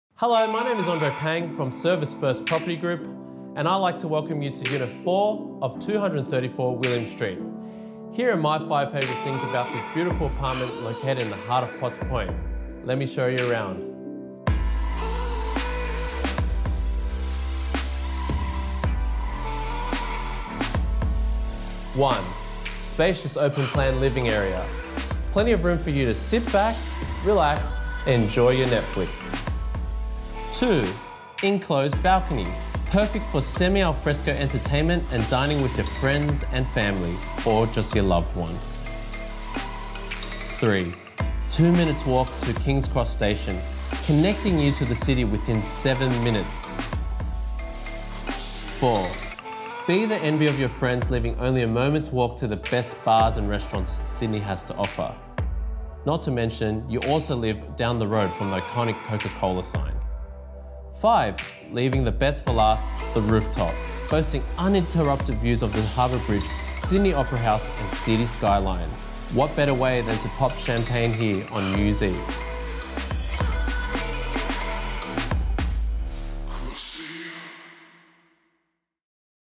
Andre's Top 5 Features of 4/234 William Street Potts Point
Executive Inner City Living with Resident Access to Spectacular Rooftop Views

Nestled in the contemporary and boutique “Banque Apartments” complex, this beautiful one bedroom apartment features spacious interiors, timber floorboards throughout and an abundance of natural light. It also boasts an extremely desirable location, with only foot steps to the iconic Coca Cola sign, Train Station, Coles and some of the best Bars/Restaurants Sydney has to offer. Residents of the boutique complex can also enjoy exclusive access to the building’s rooftop, showcasing unparalleled views of the Harbour Bridge, Opera House and Sydney’s beautiful City skyline, making this apartment a perfect starter to the inner east property market or a lucrative investment opportunity for the savvy investor.

- One spacious bedroom with built-in-wardrobes

- Large and bright living area leading out to sun-filled private loggia perfect for semi-alfresco dining

-Modern gas kitchen with sleek stainless-steel benchtop and appliances including dishwasher

-Contemporary tiled bathroom and separate internal laundry

-Rooftop terrace access for owners only, showcasing spectacular views of the Harbour Bridge, Sydney Opera House and City Skyline

-Amazing location, inner city living at the gateway to Potts Point, Woolloomooloo and Sydney CBD

-Located only 3 mins walk to Kings Cross Station and 1 min walk to Coles and the iconic Coca Cola Sign

-Surrounded by some of the best Bars and Restaurants Sydney has to offer including Cho Cho San, Apollo and Monopole

-Great opportunity to enter the inner city property market for an owner occupier or an investor wanting fantastic potential on returns